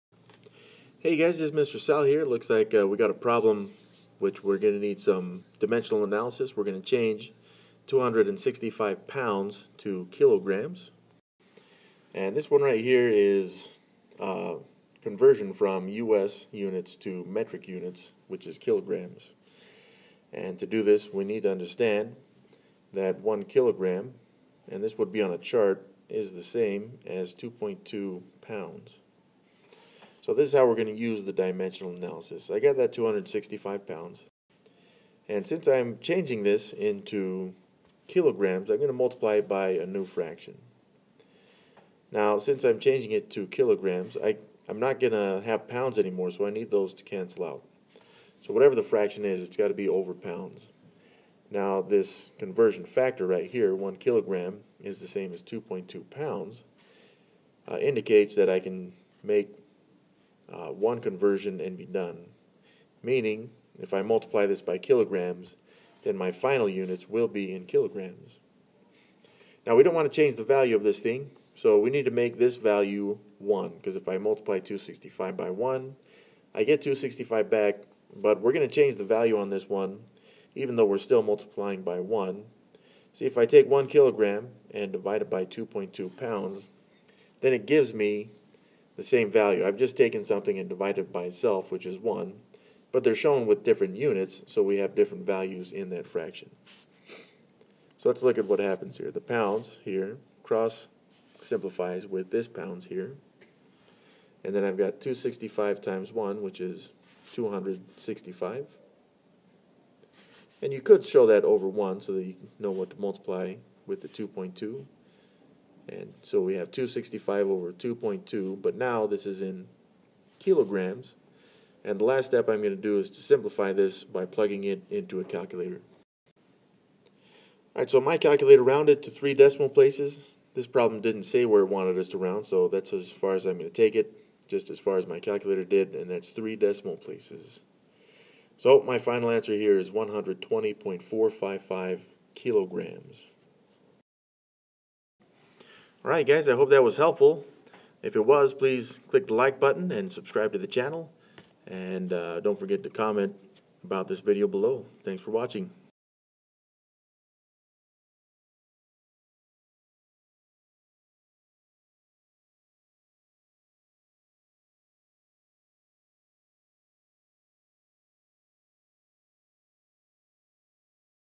How to Convert 265 Pounds to Kilograms using Dimensional Analysis
For this problem we learn how to convert 265 pounds to kilograms using dimensional analysis.